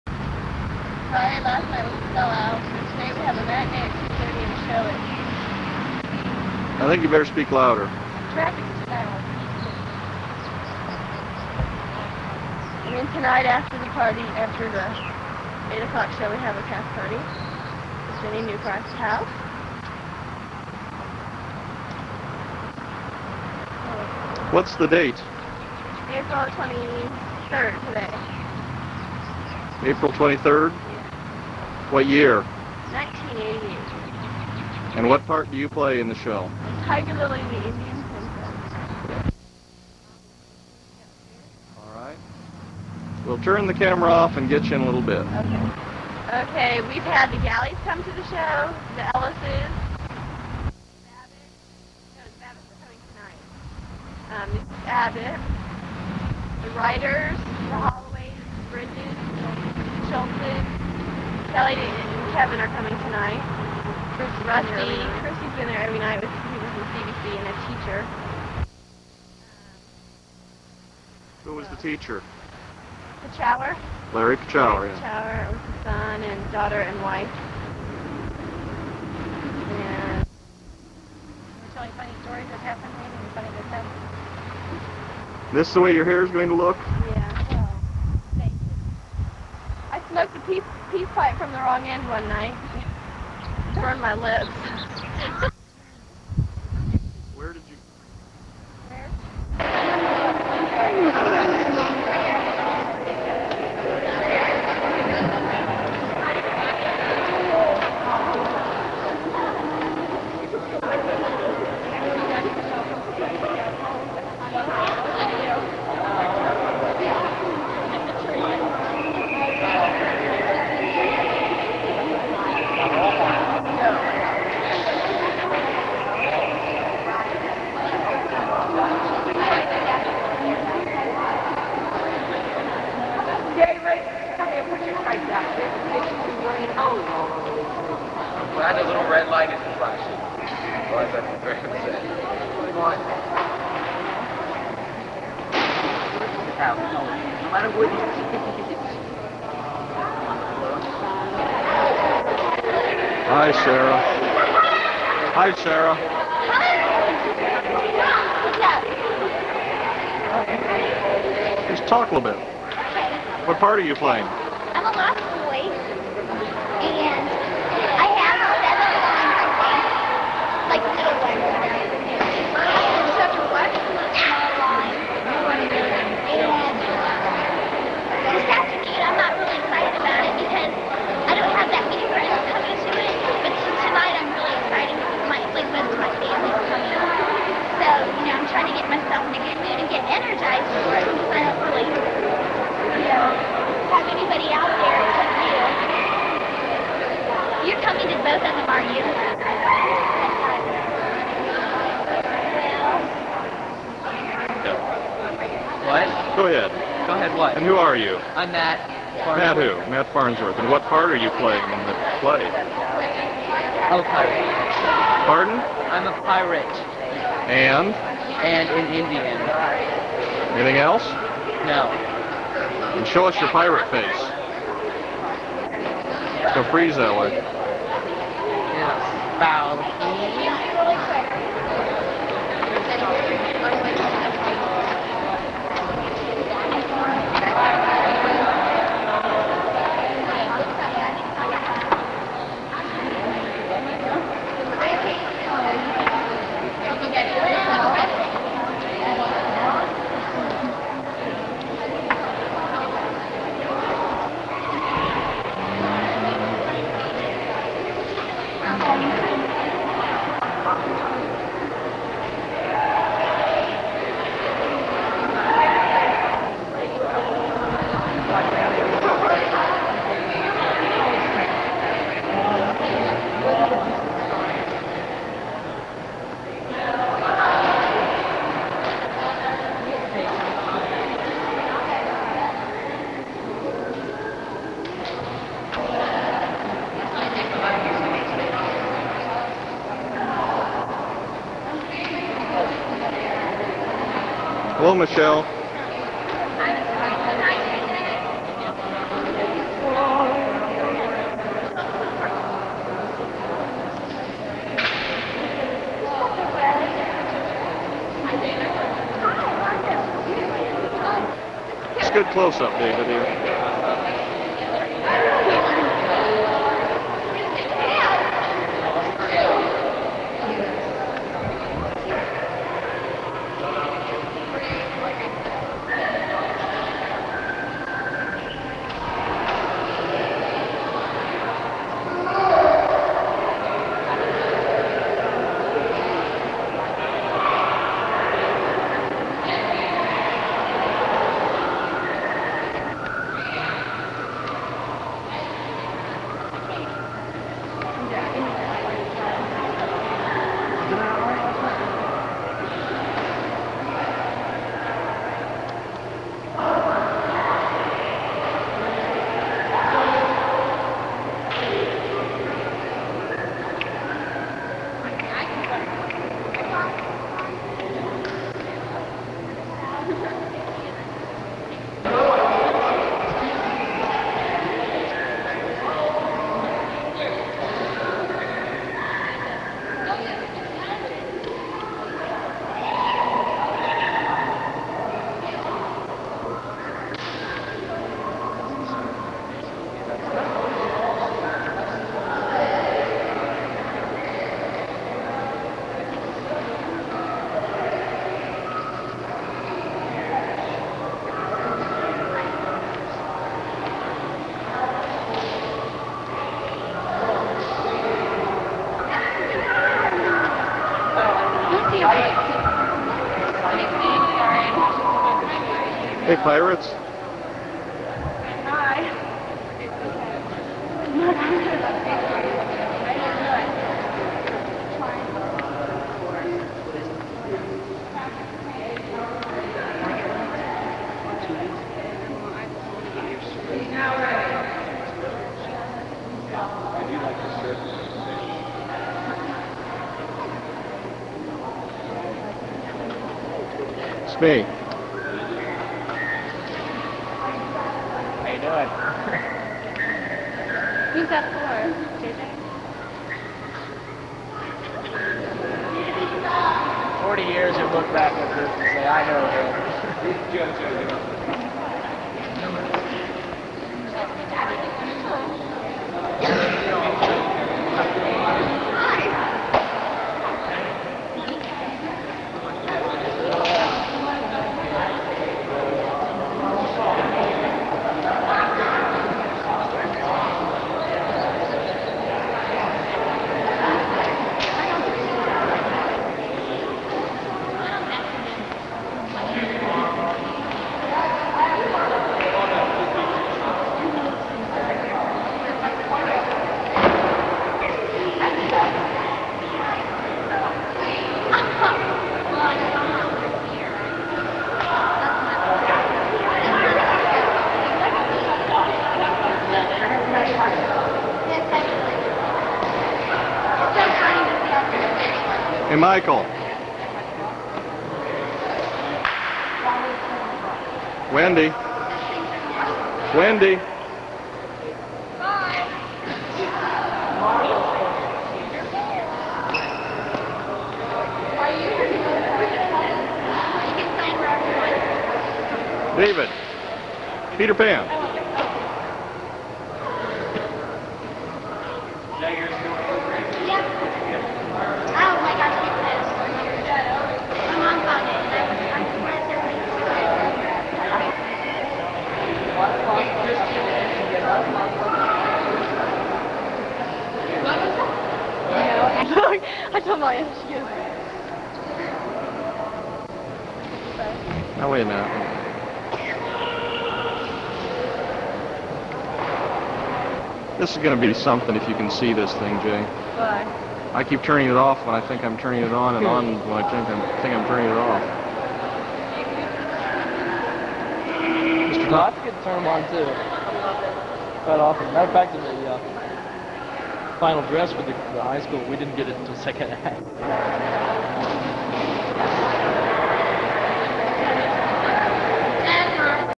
SHS Spring Musical (1988) - Candid Backstage Footage
Comments before the show by JJ Mason Taylor (aka Tigerlily) plus footage shot in the Sycamore Junior High gym.

Glimpses of Leslie Knotts and Denny Thomas.